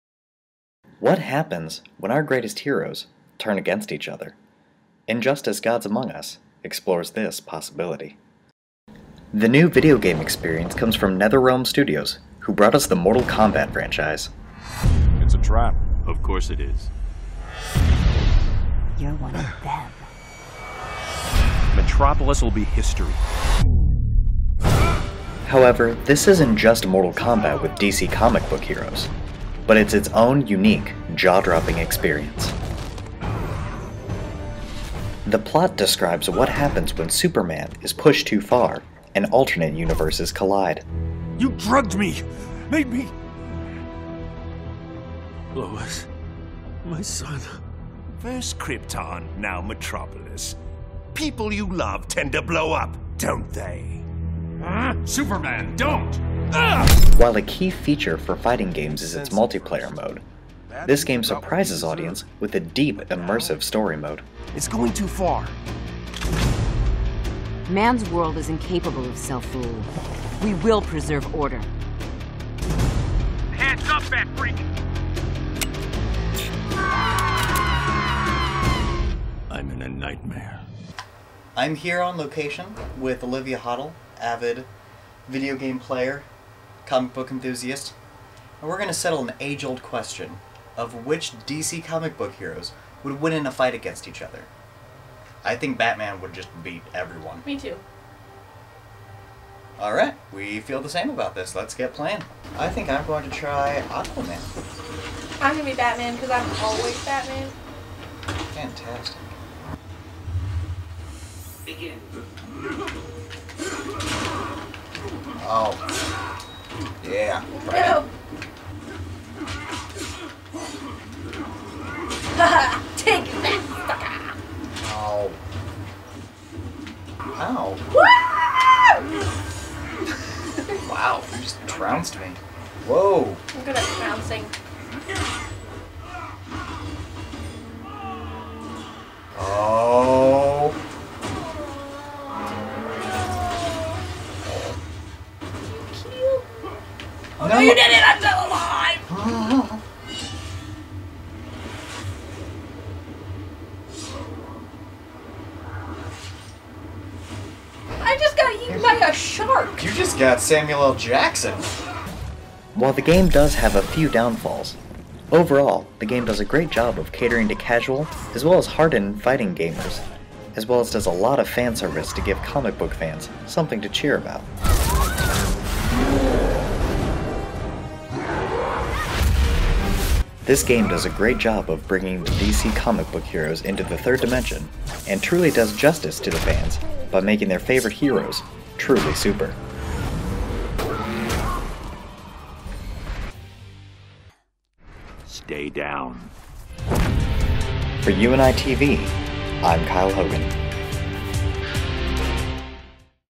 UNI-TV Injustice Review
Kyle Hogan looks at the latest video game engaging comic book fans and gamers alike.